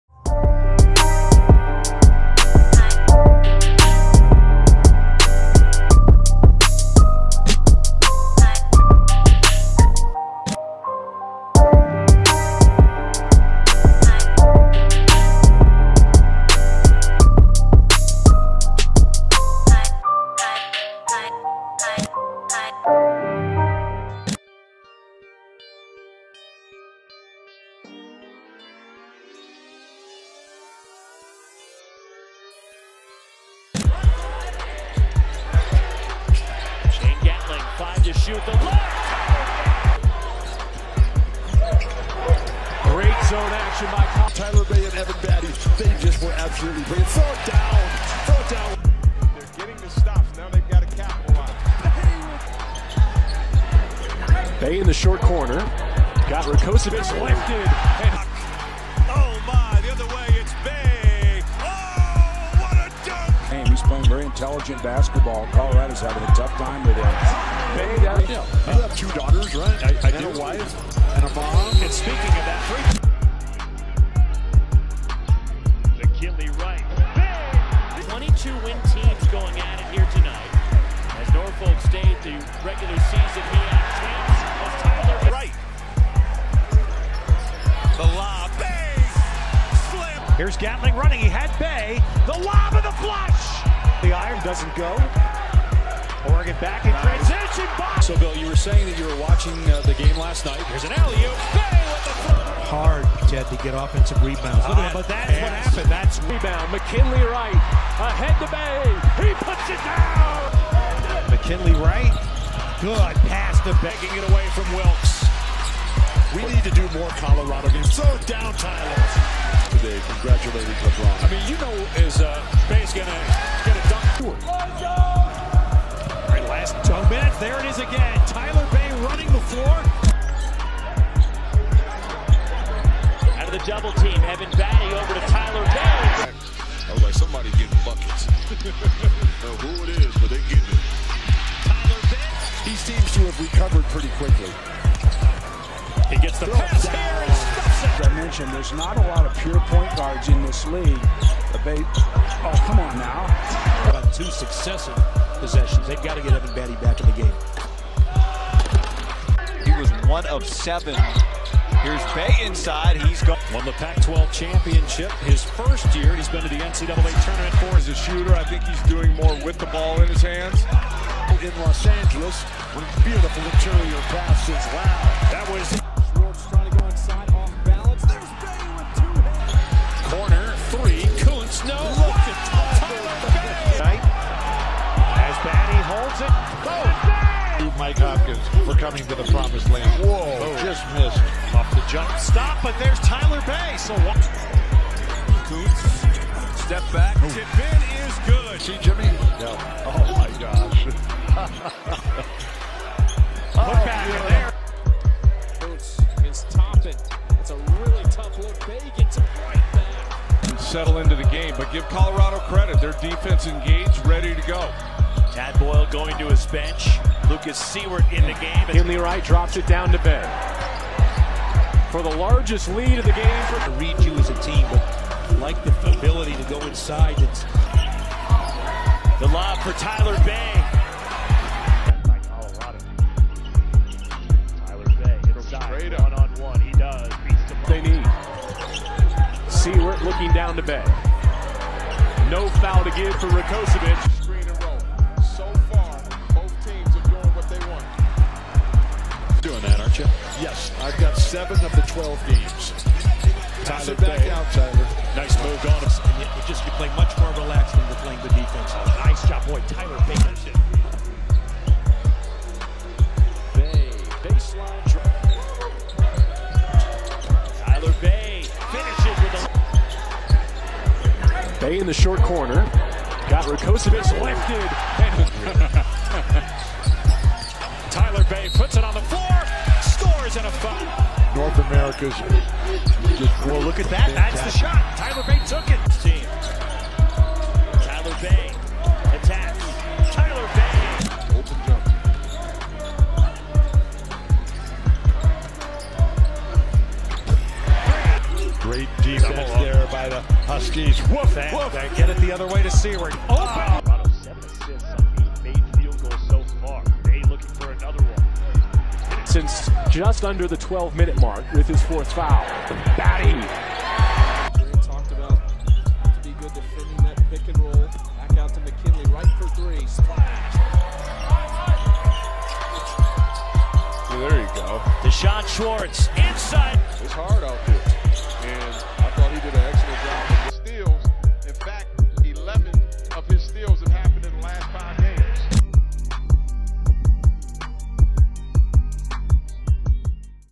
2020 NBA Draft Junkies Profile | Tyler Bey | Offensive Strengths
Offensive strengths highlights from Tyler Bey's sophomore season at the the University of Colorado.

Bey jumped on NBA radar in in his sophomore seasons in which he  averaged 13.6 ppg and 9.9 rebounds while earning  first team All-Pac-12 honors and winning the league’s Most Improved Player award.